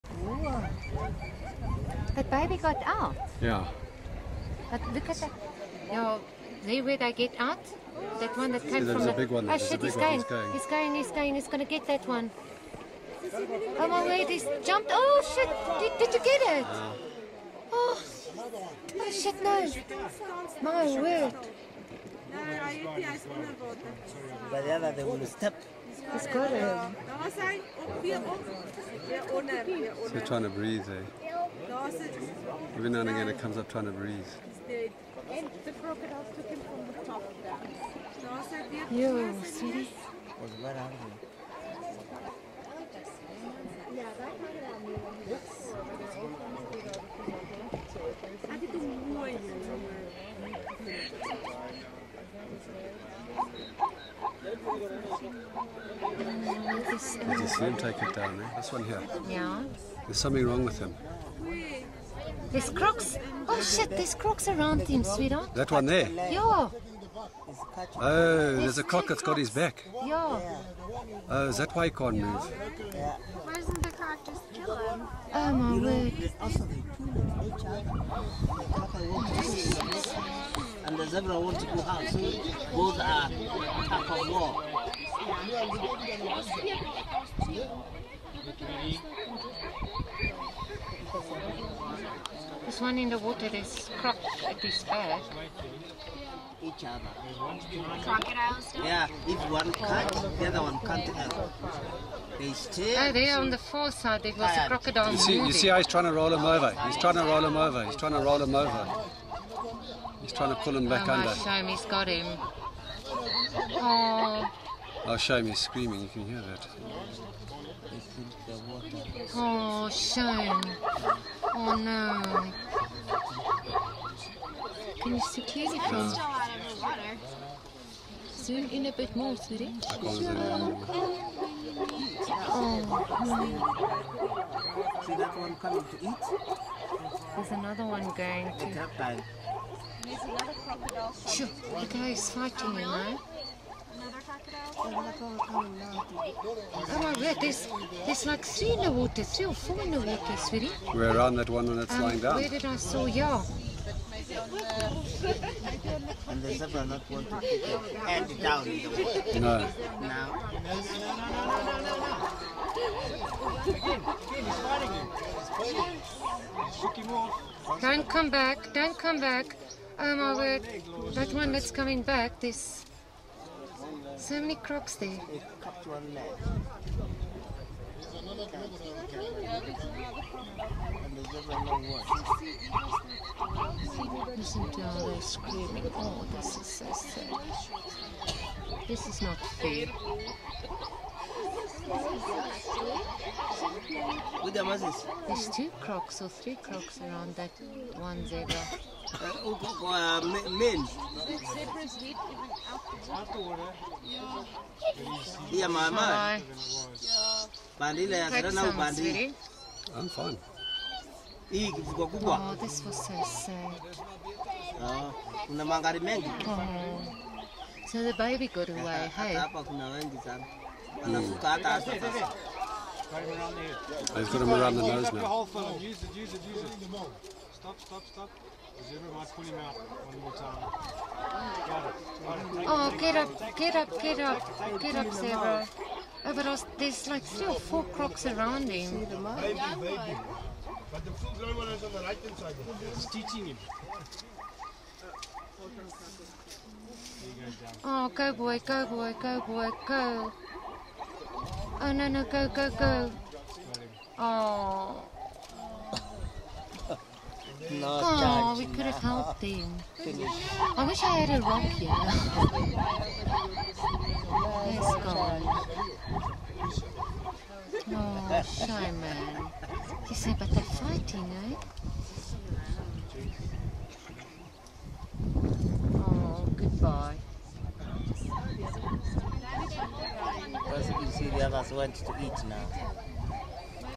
Massive Crocodile grabs zebras
During the great migration crocodiles take out as many wildebeests and zebras as they can so as to sustain them for the rest of the year. The mara crocodile are some of the biggest crocodiles in the world and the biggest nile crocodiles.

Filmed in Masai Mara

Maasaisightings.com